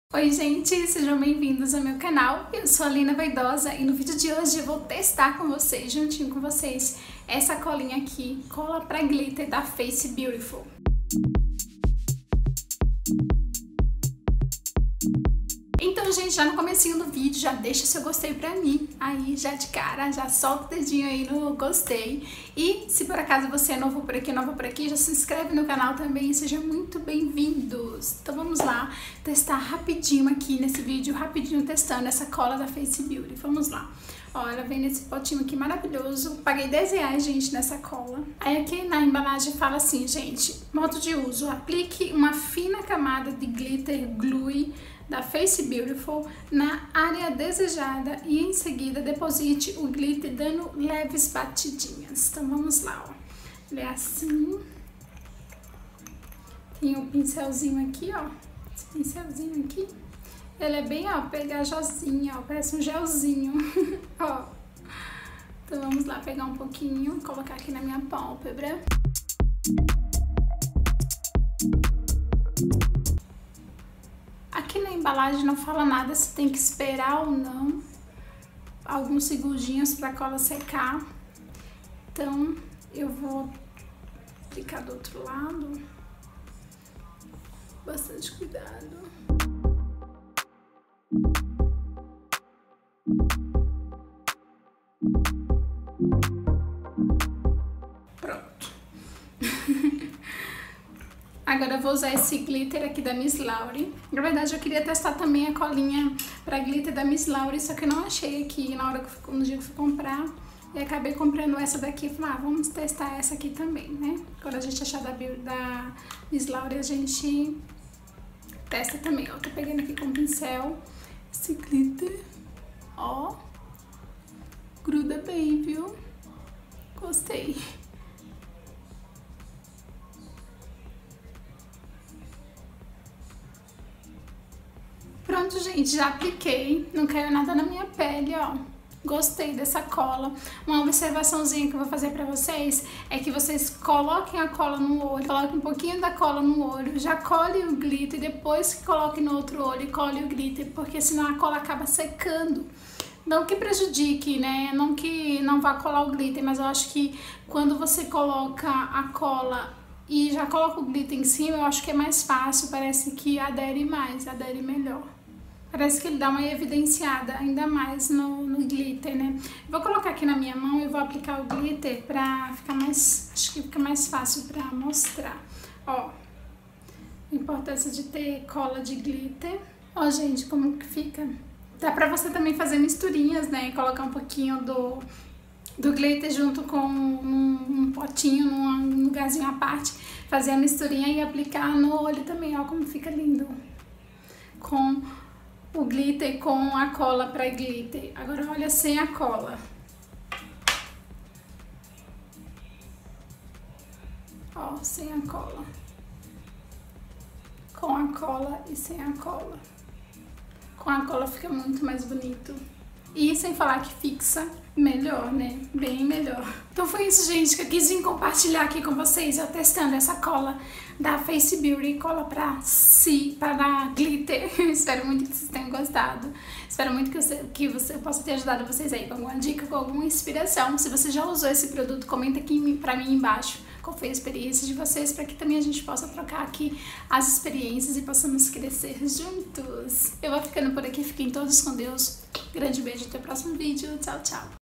Cola Para Glitter Face Beautiful - Testando | Lina Vaidosa
Oi, gente linda! Hoje eu vim testar com vocês a cola para glitter da Face Beautiful. Espero muito que gostem! Beijo 😘